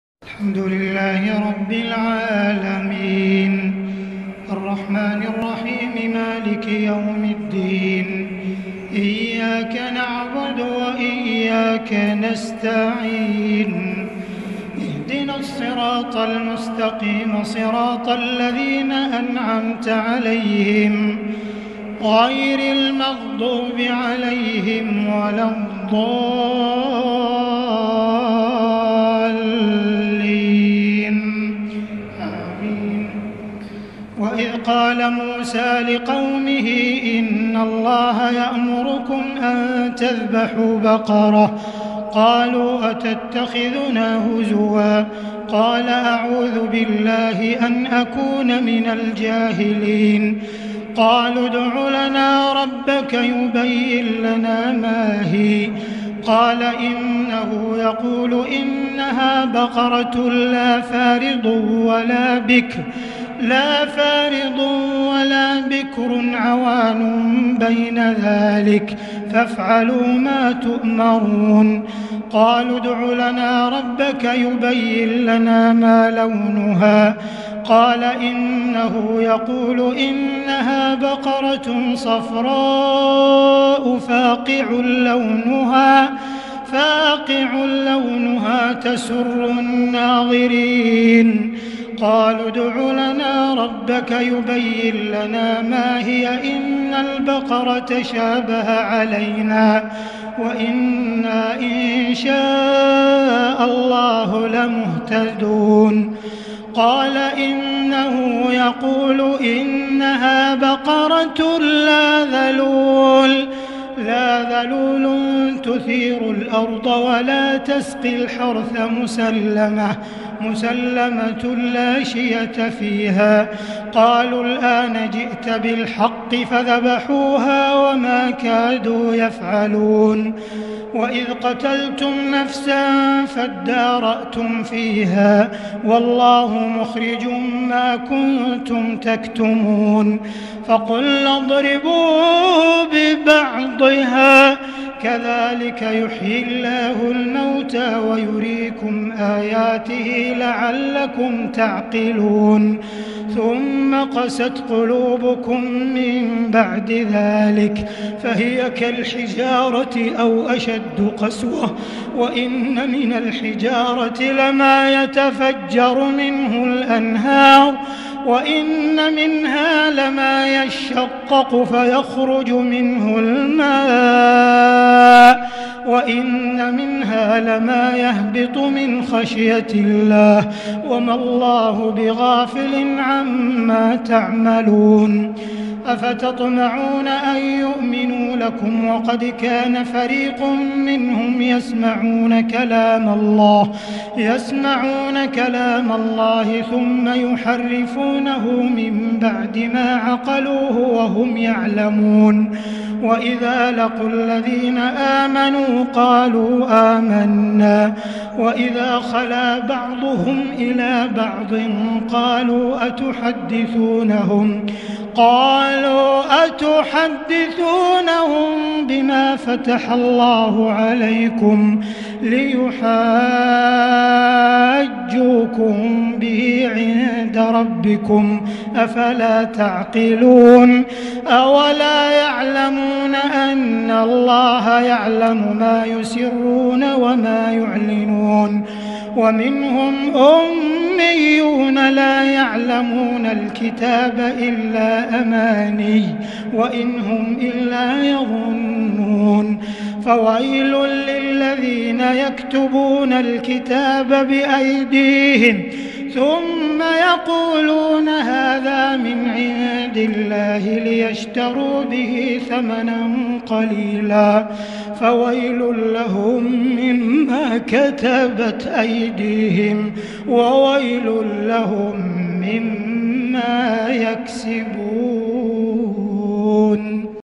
1st Ramadan 1441 Makkah Taraweeh Sheikh Abdul Rahman Al-Sudais 23 April 2020
New Video 1st Ramadan 1441 Makkah Taraweeh Sheikh Abdul Rahman Al-Sudais 23 April 2020 PAKURBAN